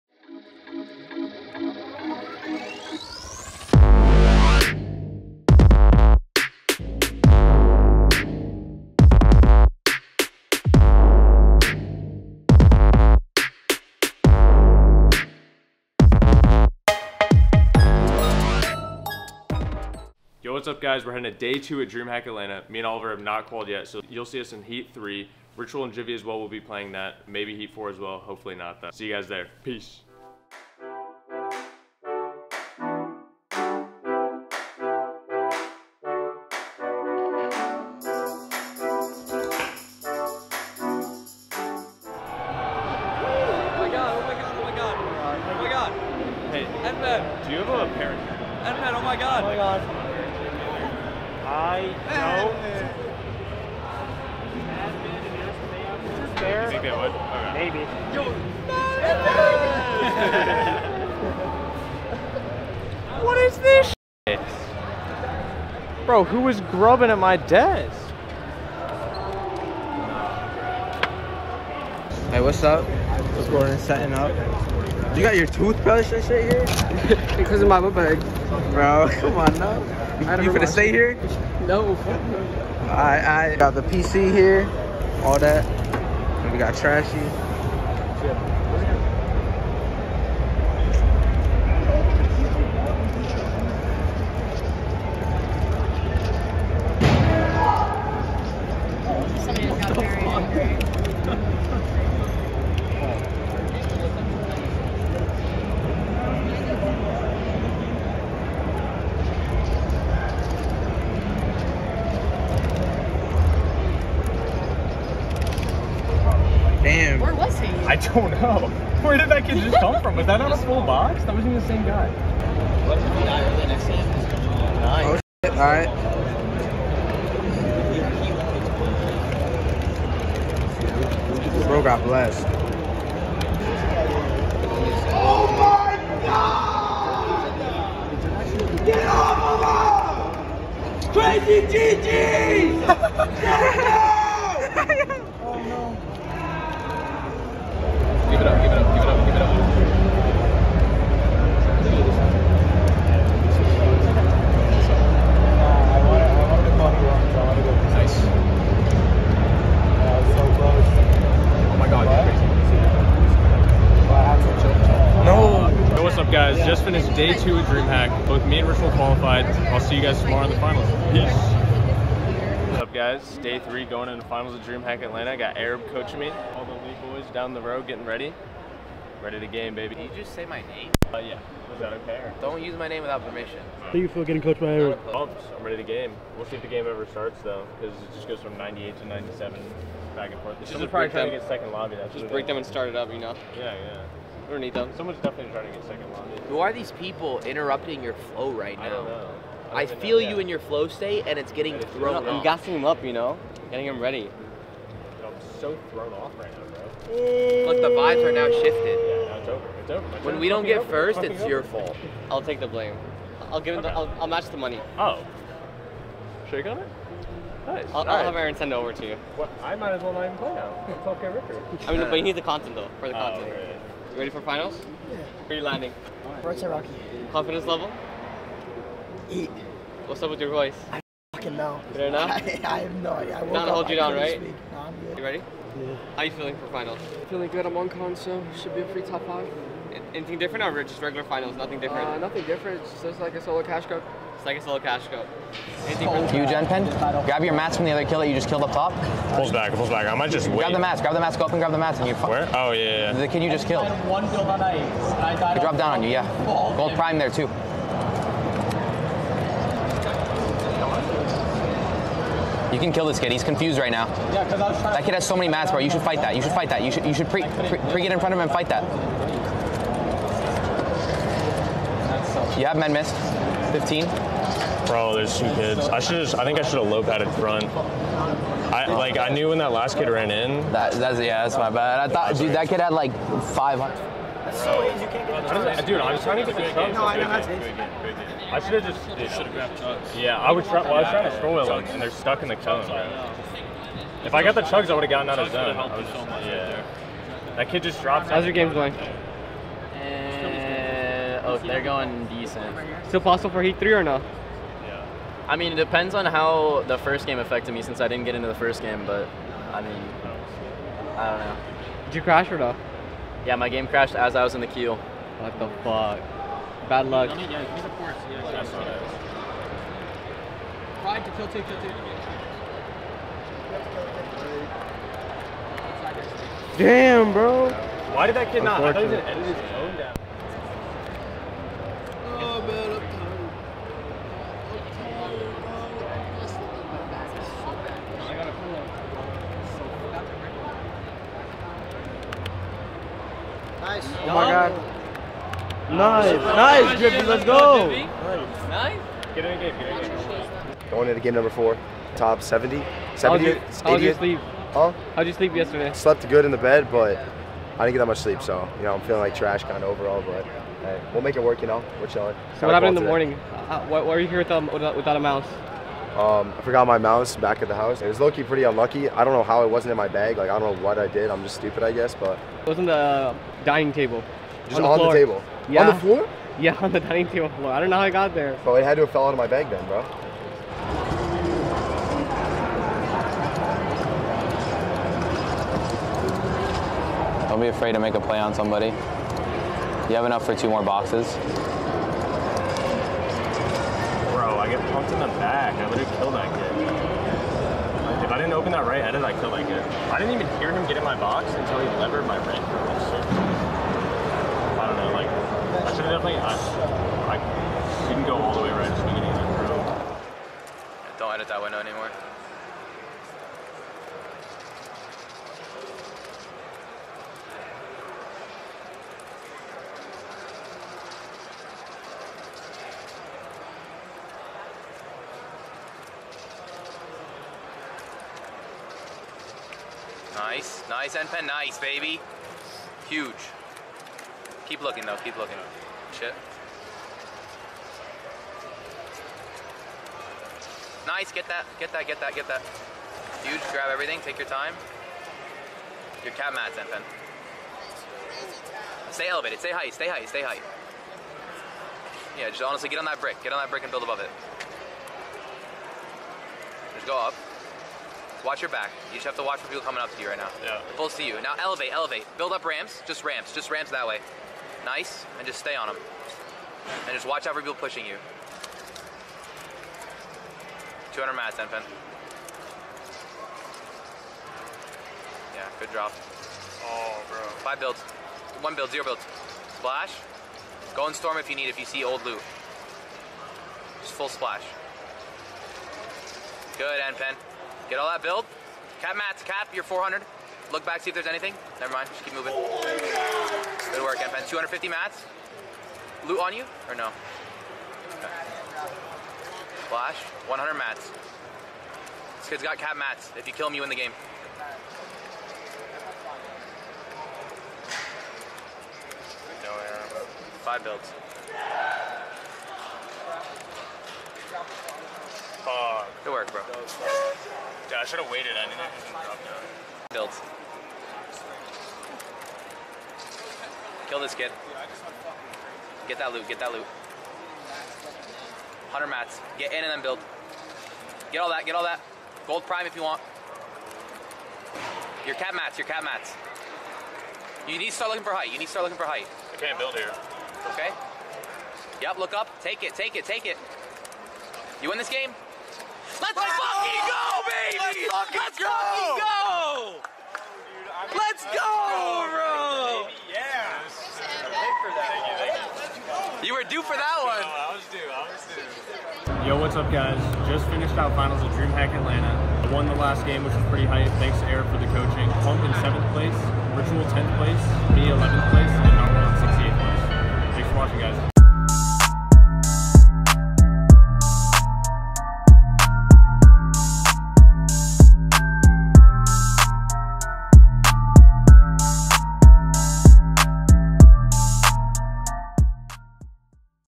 Fortnite DreamHack Atlanta Vlog #2 (ft. Npen, Pxmp, OliverOG, Arab and MORE!)
In this video, Elite took over DreamHack Atlanta, Pxmp placing 7th, Ritual placing 10th and npen placing 11th!  Couldn't be more proud of our guys!  We had a great time in Atlanta watching our Fortnite Professional Players compete!  We are excited for the future of LAN events!